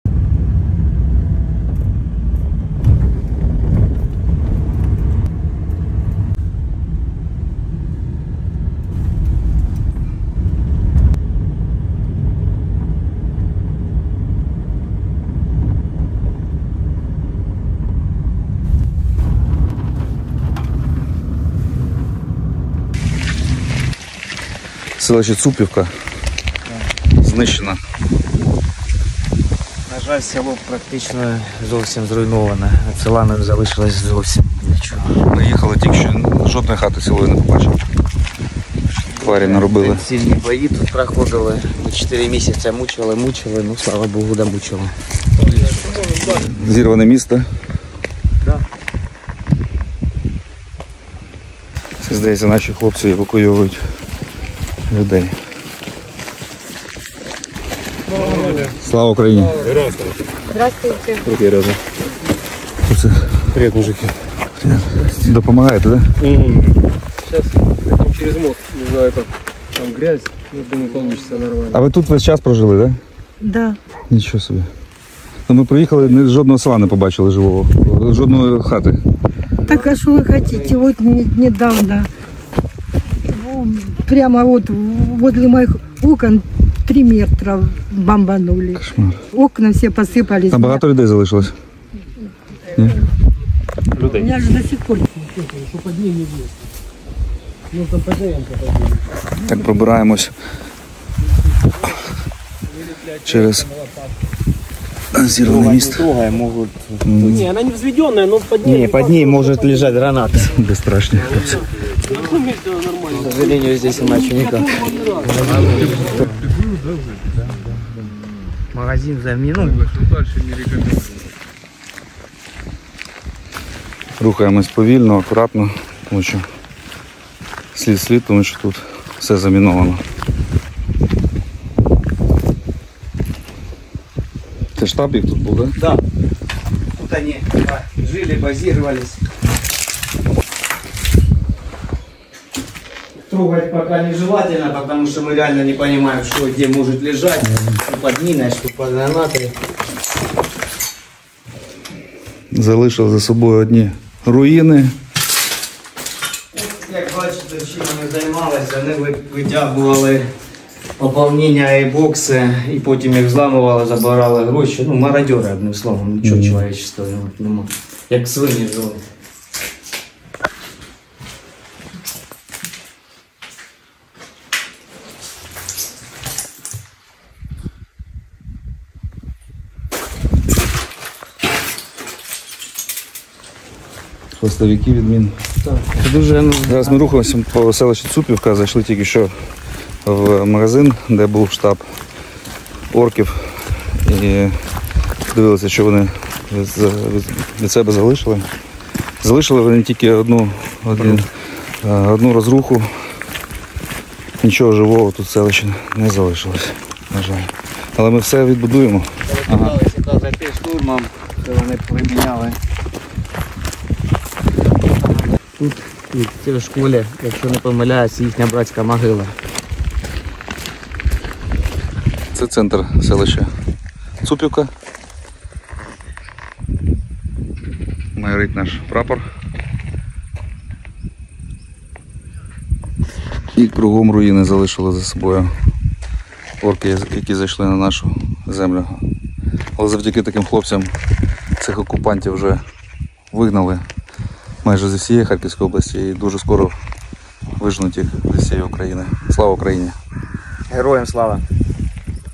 Не вціліла жодна хата. Російській окупанти повністю знищили с.Цупівку Харківської області.
Перевірили місцевість разом з нашими військовими.
Територія села замінована.
Знаходитися тут небезпечно.
Оглянули приміщення місцевого магазину в якому ворожі війська облаштували собі штаб. Руїни і смерть - все, що вони тут по собі залишили.
Все відновимо!
Але не пробачимо!Разом до Перемоги! 🇺🇦Шана і вдячність захисникам!